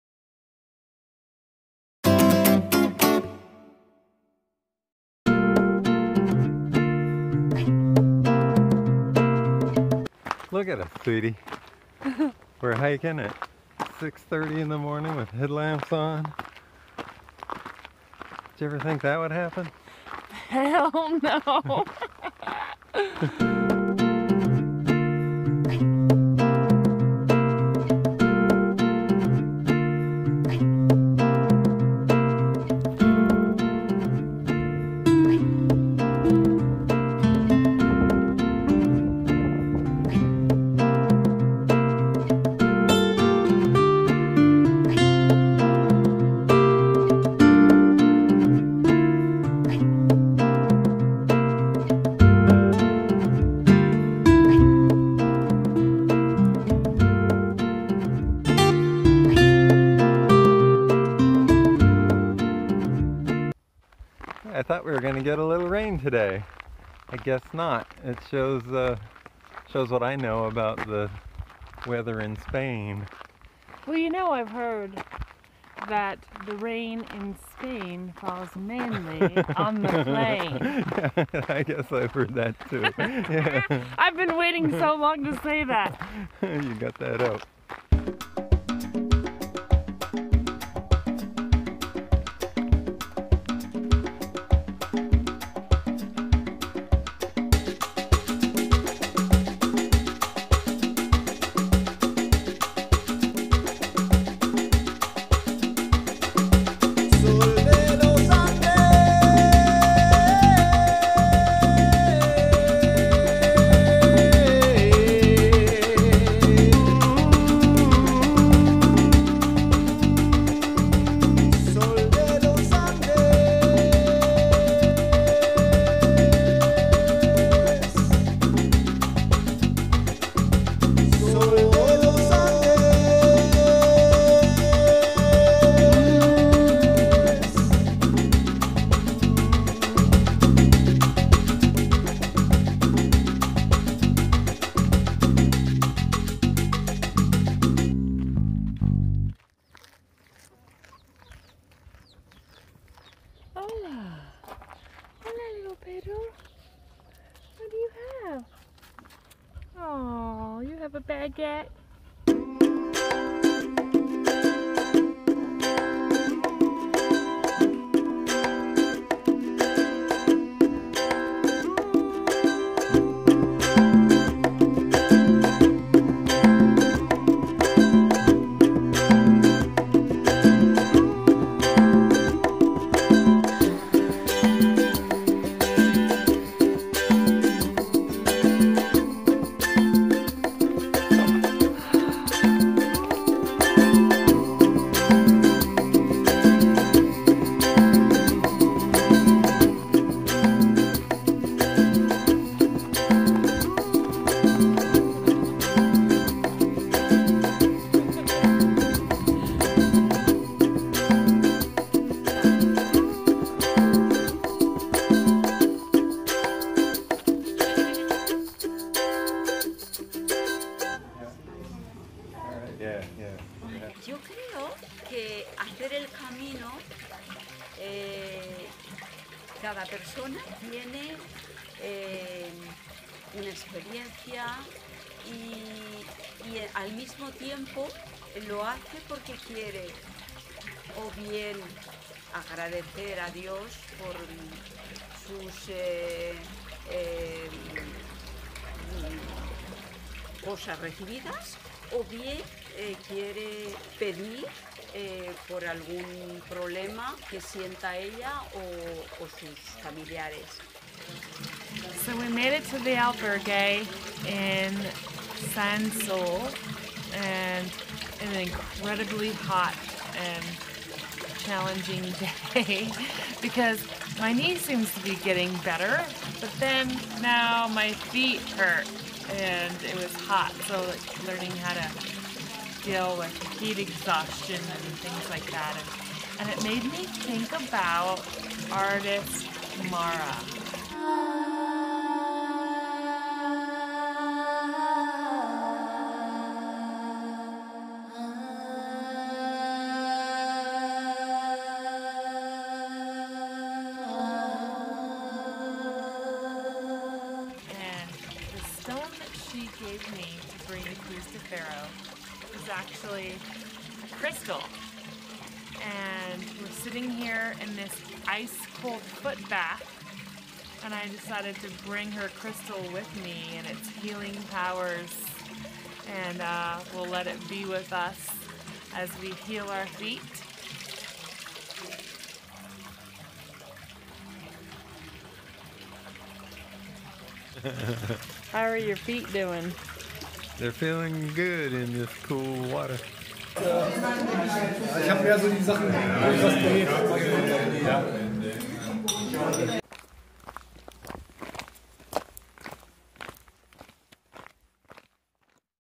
Day 10 - Azqueta to Sansol
Follow John and Cindy as they hike The Camino de Santiago.

original music by Nina Pollock